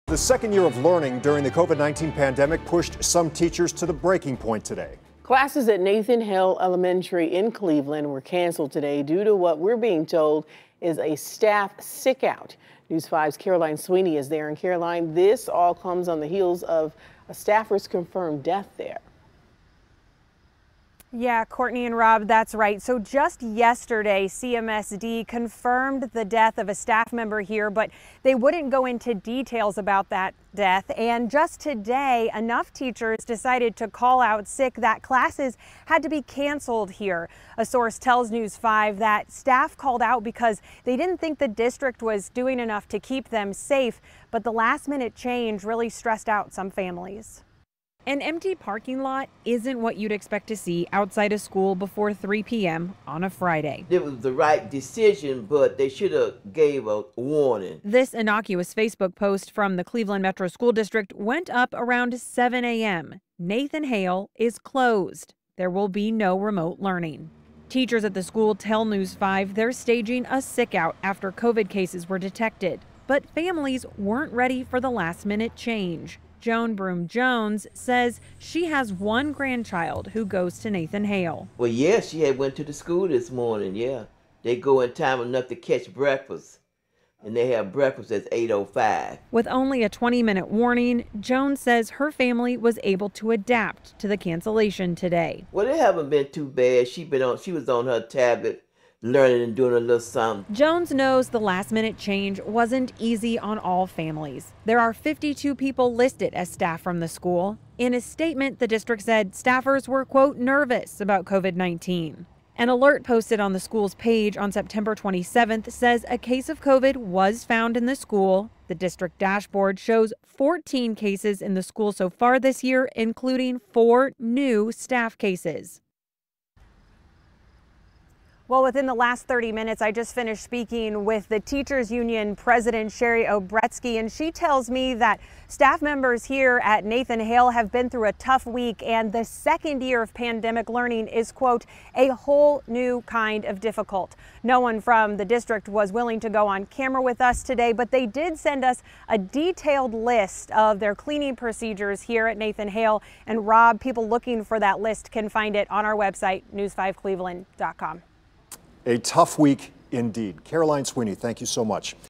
Nathan Hale Elementary closed Friday due to staffing shortage
Classes were canceled Friday for students at Nathan Hale Elementary due to a staffing shortage.